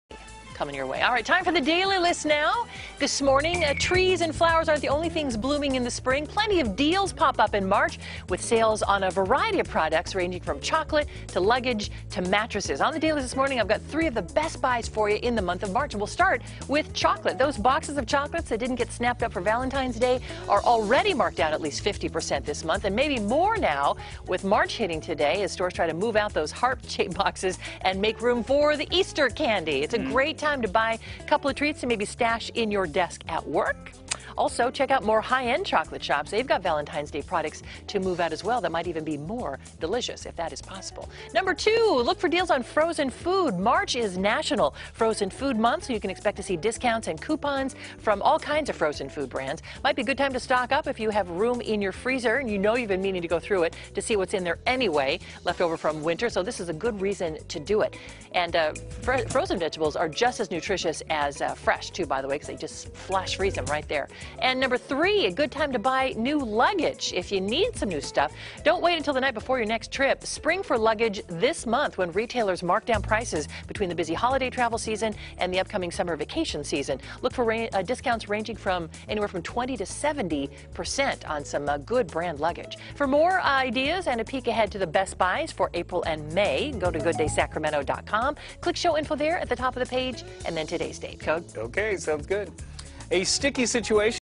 Daily List: Best Buys for March
On the Daily List, Marianne is giving you three best buys for March.